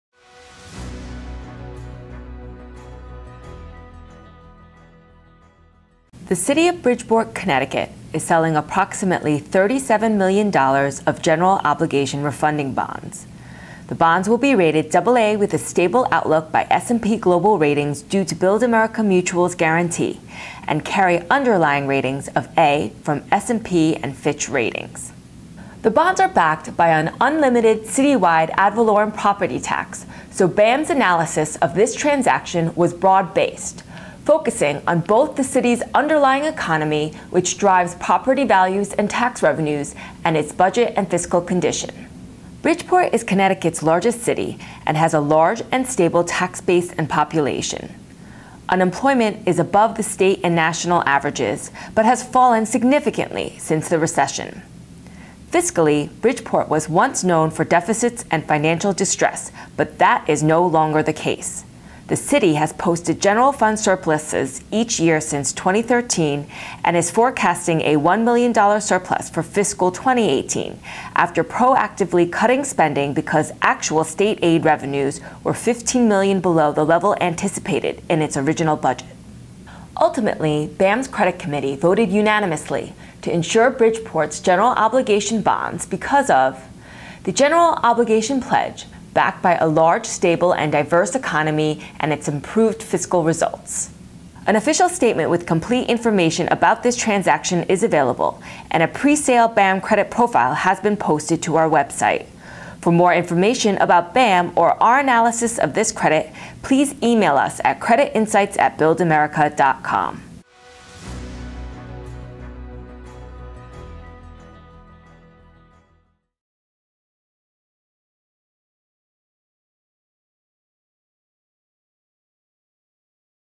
What's Changed in Bridgeport? A lot.
Bridgeport is the largest city in Connecticut and was once known widely for its fiscal distress. Now, after posting five consecutive annual budget surpluses (and anticipating a sixth in 2018), the city is preparing a general obligation refunding bond issue.  Build America Mutual's Juliet Stiehl explains the analysis that led to BAM's decision to guarantee the bonds.